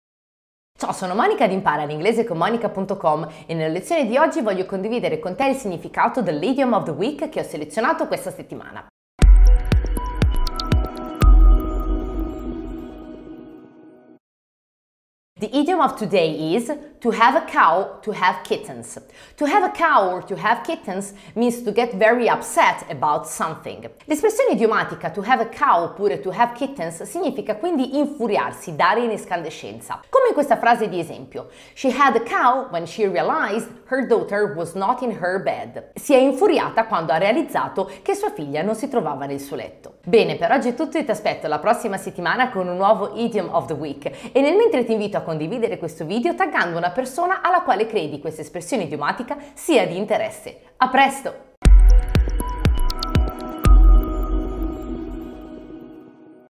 TO HAVE A COW / KITTENS: English Idiom
[TO HAVE A COW / KITTENS]
Qual è il significato di questo IDIOM?
Cosa si intende quando si dice “to have a cow / kittens” in inglese?
Guarda ORA il mio video e lo scoprirai! Ti aspetto venerdì prossimo con una nuova espressione idiomatica che ti aiuterà a rendere il tuo inglese “more natural”!

Riceverai per 4 giorni 1 lezione di estremo valore direttamente sulla tua email, a costo zero!